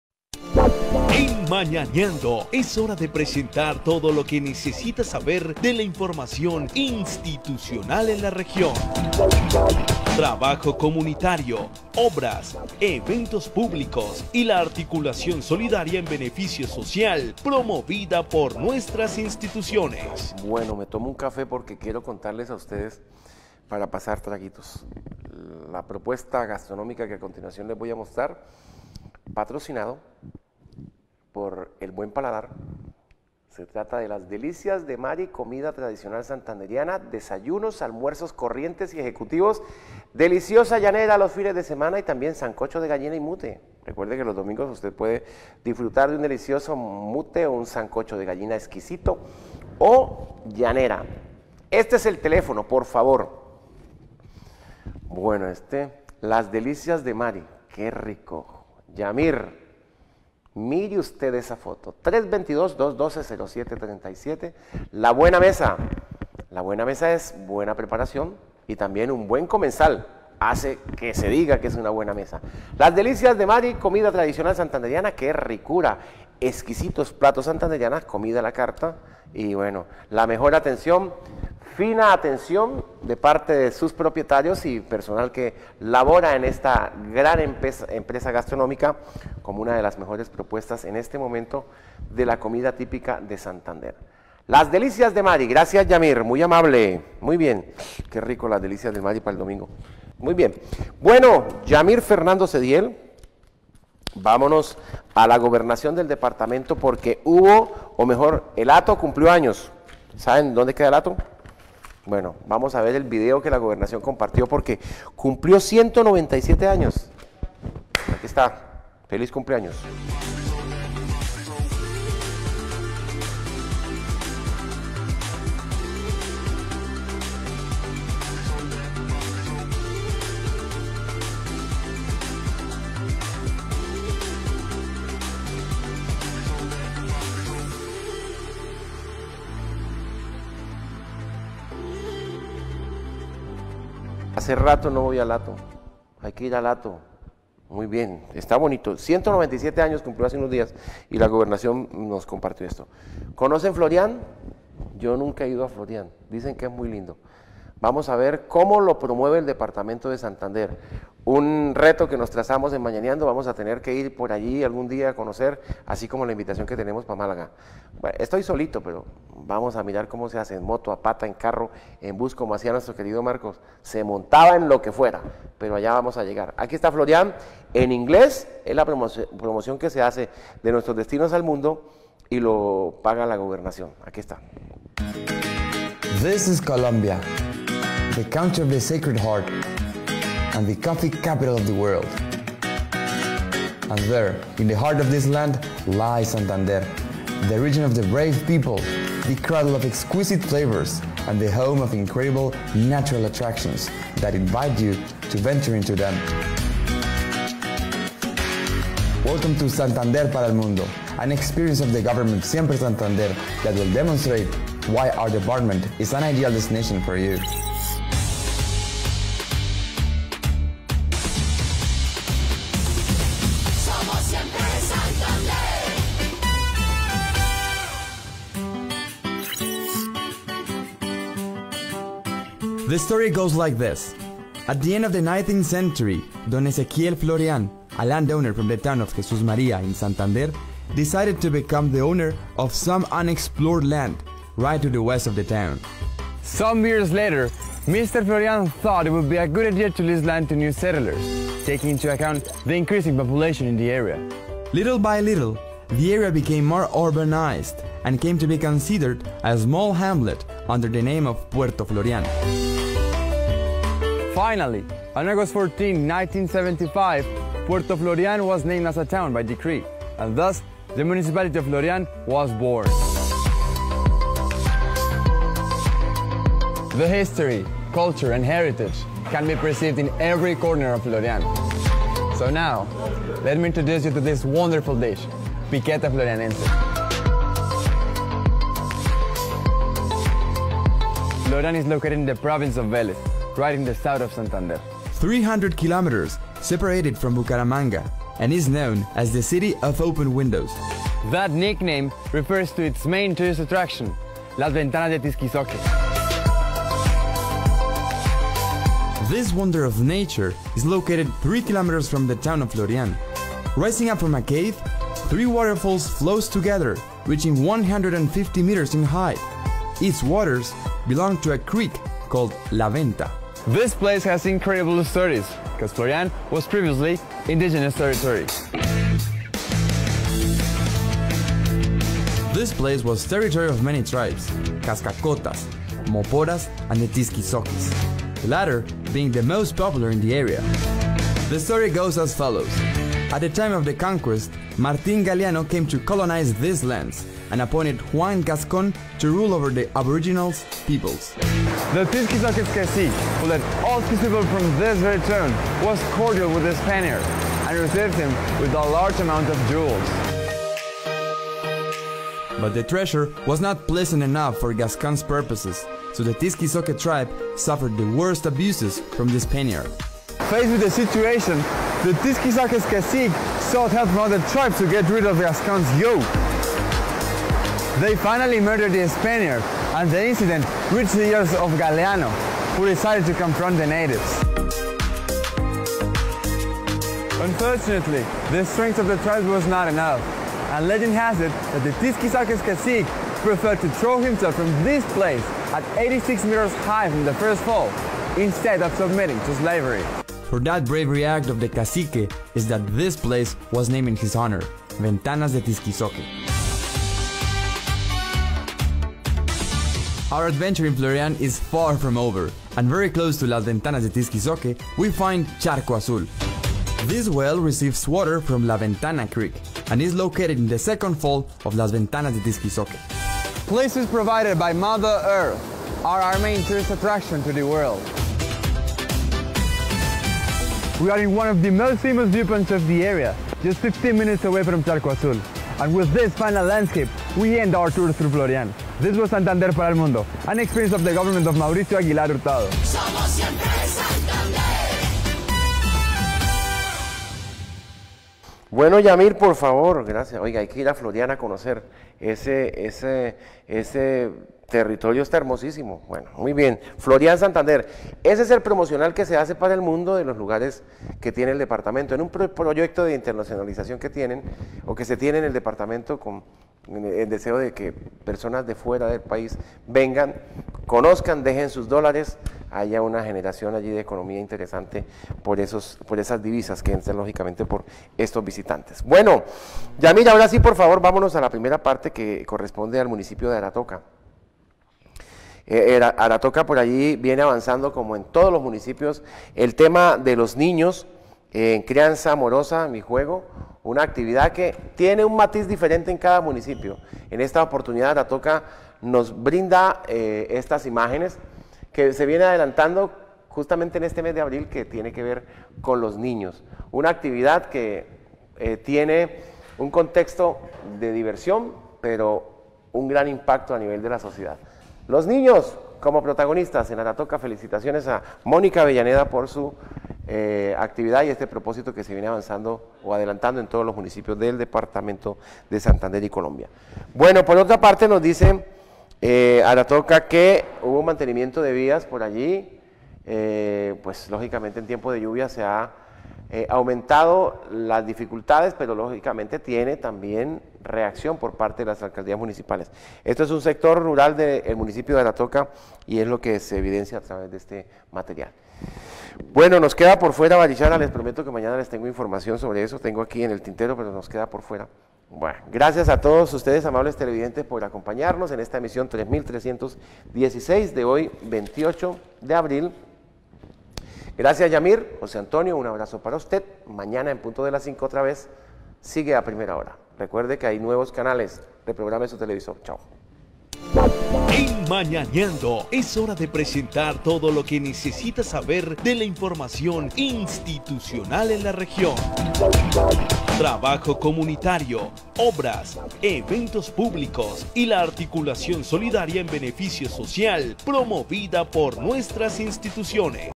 UN RECORRIDO POR ARATOCA, HATO Y FLORIAN EN SANTANDER
Con la presentación de Jairo Navarro Jaimes conozca algunas novedades de estas municipios en Colombia.